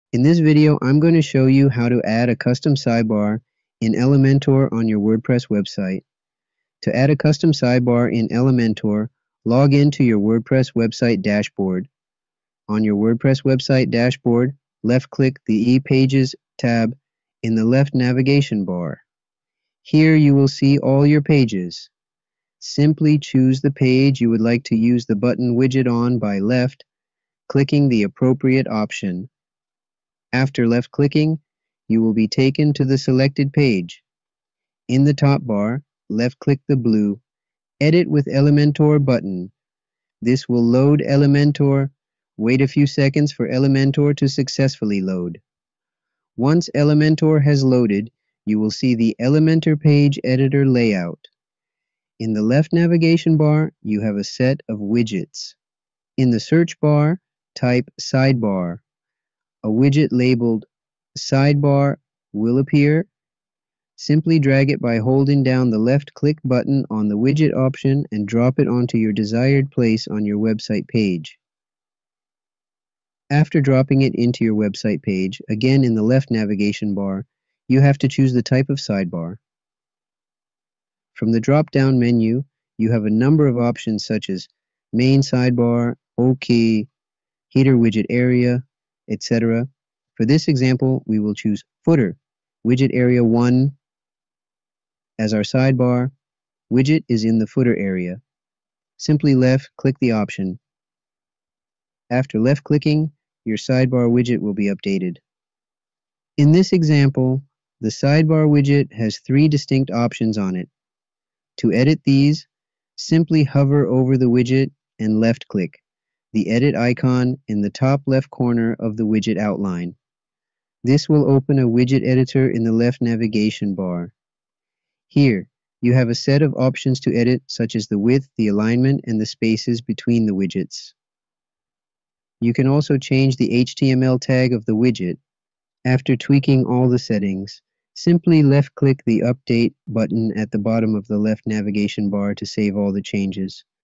How to Add Custom Sidebar in Elementor WordPress (2024 Update)
Learn how to enhance your Elementor WordPress website with a custom sidebar in this step-by-step tutorial! In this 2024 update, we'll guide you through the process of creating and integrating a personalized sidebar using Elementor, empowering you to design your site exactly how you envision it. Whether you're a beginner or an experienced user, you'll find this tutorial easy to follow and implement. Watch now and take your website customization to the next level!